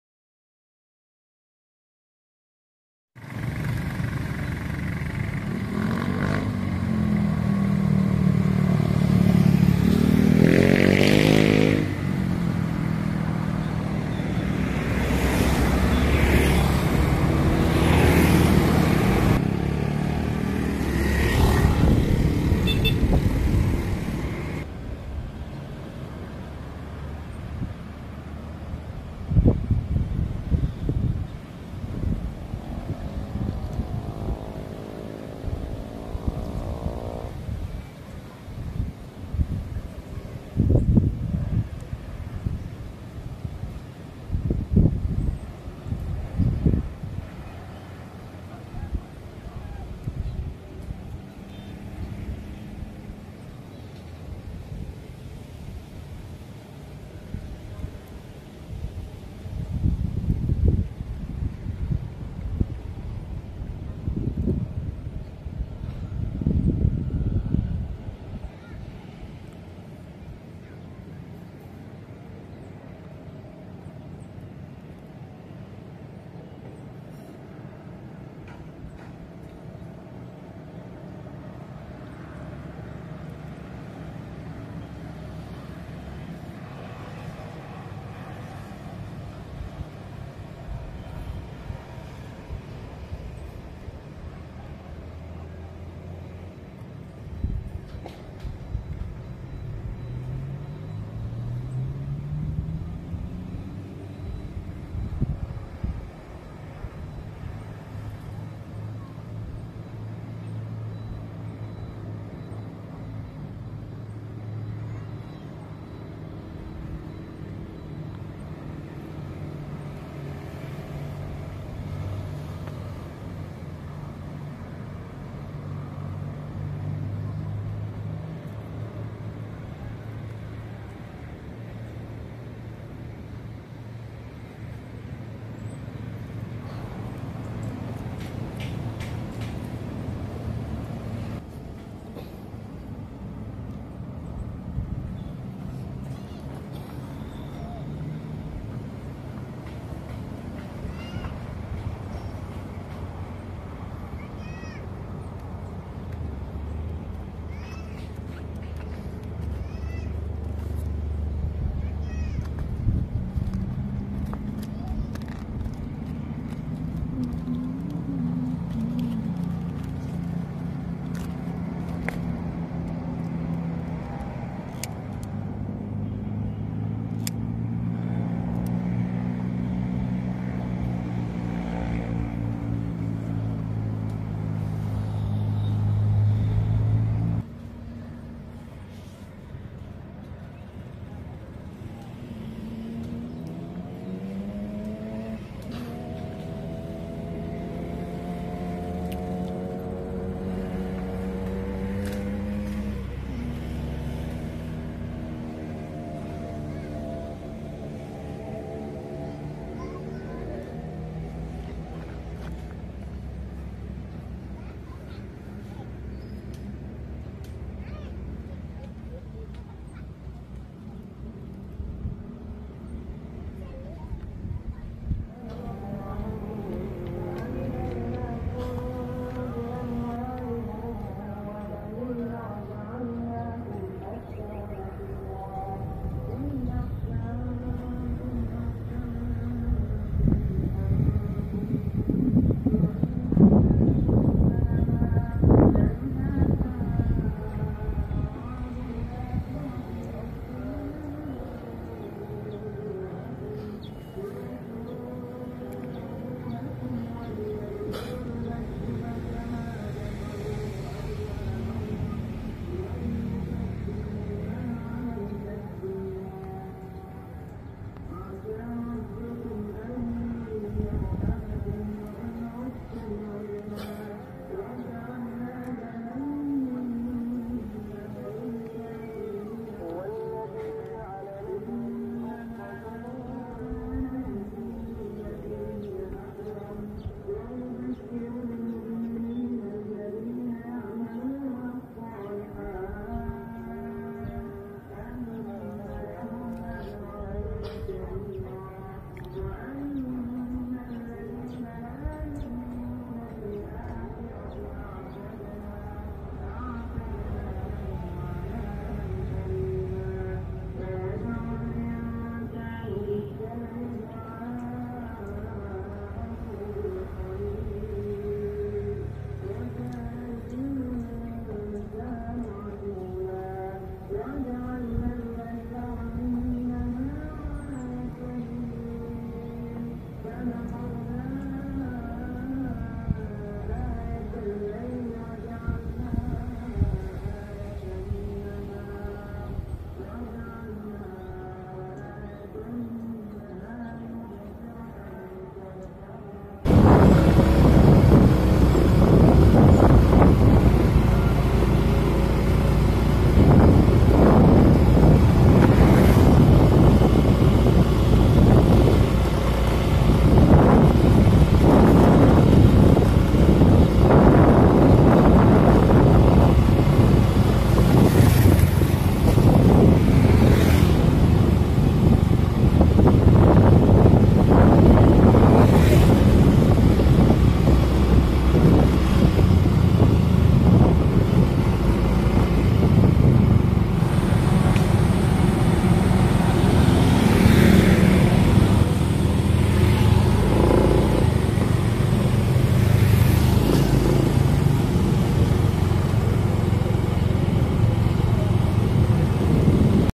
Benteng Kesultanan Buton Terluas di Dunia Berdiri di Kota Baubau pada abad ke 16 hingga abad ke 20.
Benteng Kesultanan Buton Terluas di Dunia Berdiri di Baubau pada abad ke 16 hingga abad ke 20, dan benteng ini berada di provinsi Sulawesi tenggara, Indonesia
Benteng Kesultanan Buton Terluas di Dunia Berdiri di kota Baubau pada abad ke 16 hingga abad ke 20#bekasberadabansukuButon